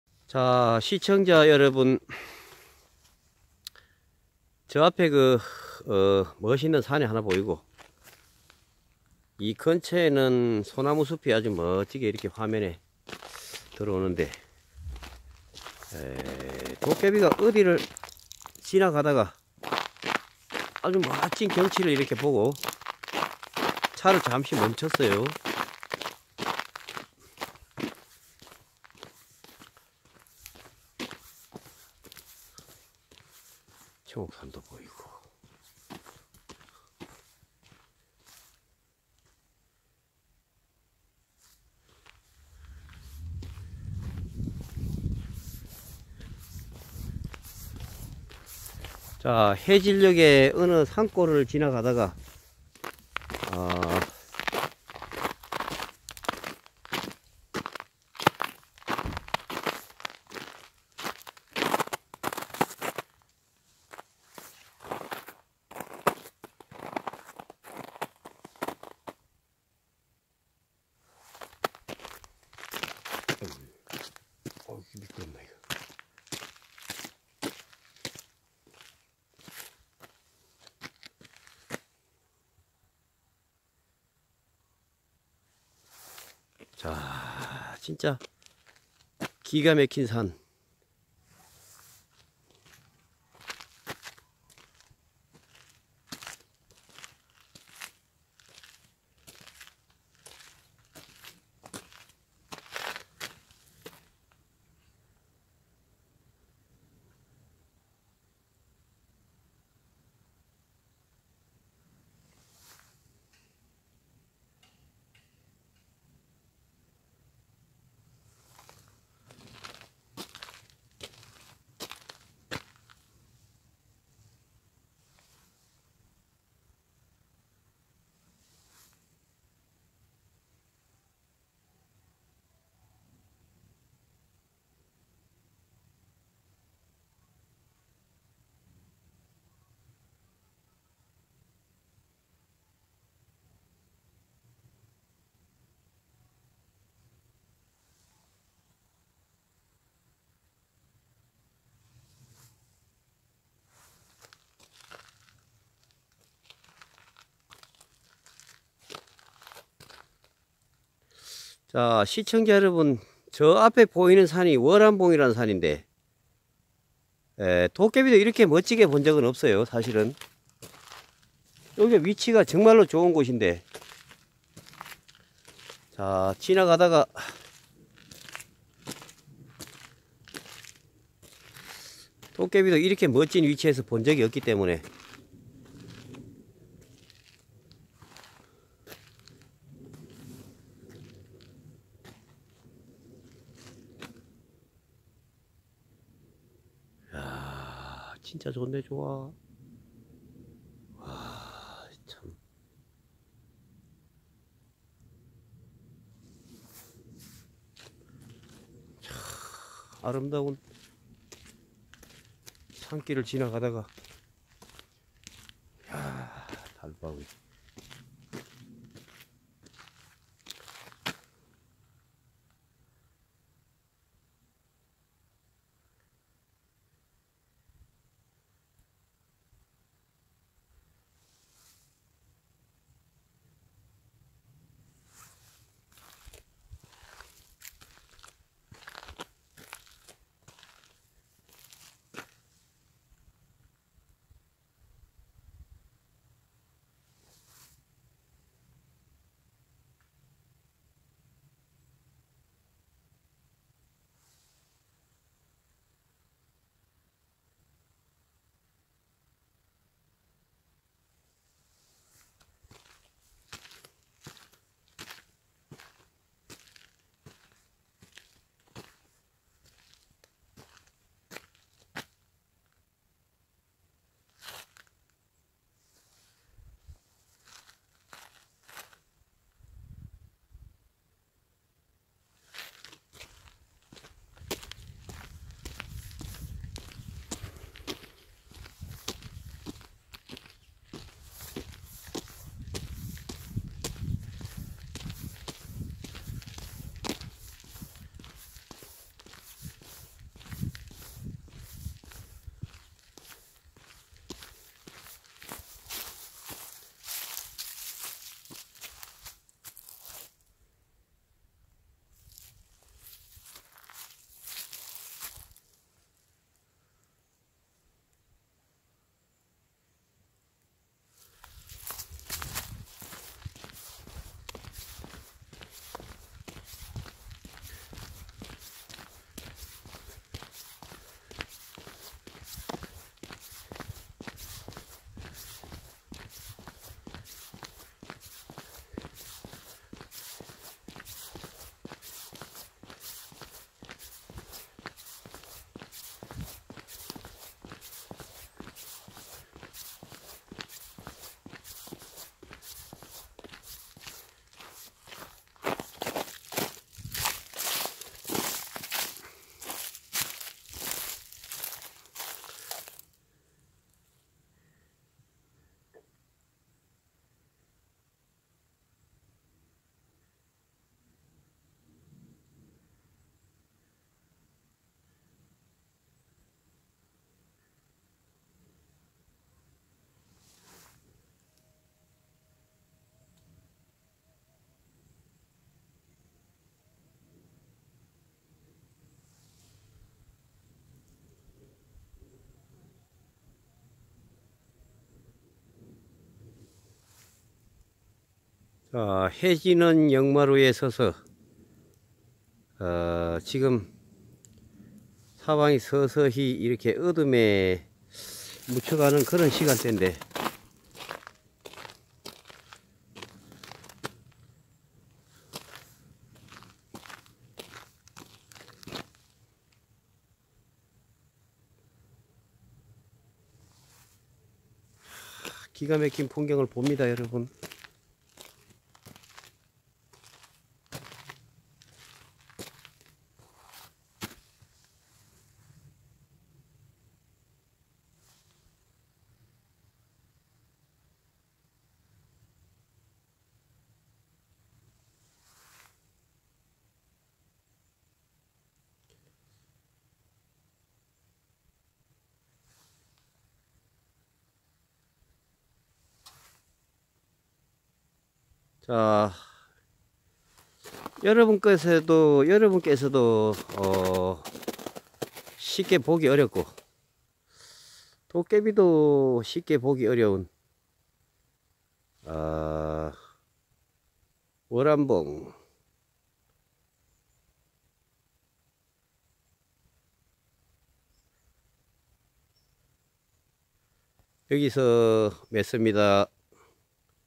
산골을 이동중 달바우 보고야 말았다 말로만 들었던 충격적? 경치는 ??
그동안 달바우(월암봉)어디서 보아야 제데로 볼란가
위치를 찾지못해 헤메었다 비로소 제대로 관찰했다!